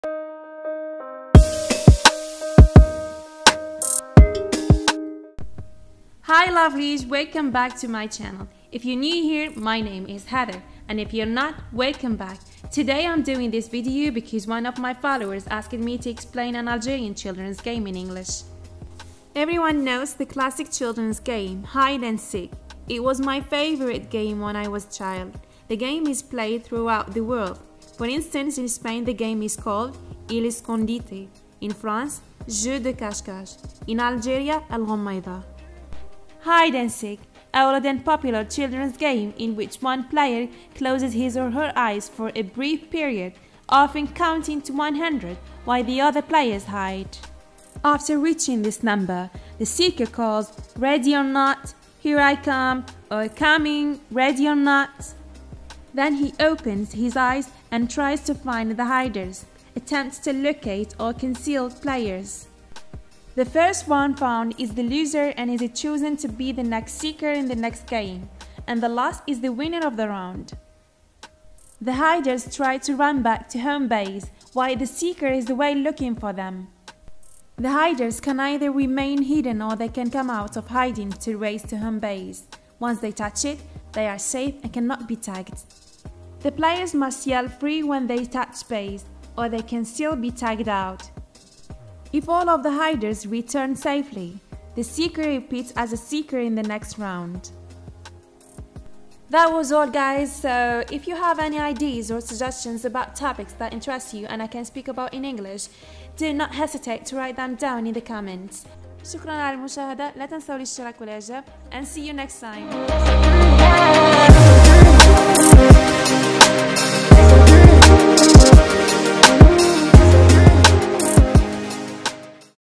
Hide and seek / الغميضة كيف تعبر عن لعبة اطفال جزائرية بالإنجليزية
Algerian children's game
Hide and seek
لعبة جزائرية قديمة
الغميضة
تعلم الإنجليزية مع هيذر
كيف تعبر عنعن لعبة جزائرية قديمة بالانجليزية
كيف تعبر بالانجليزية الغميضة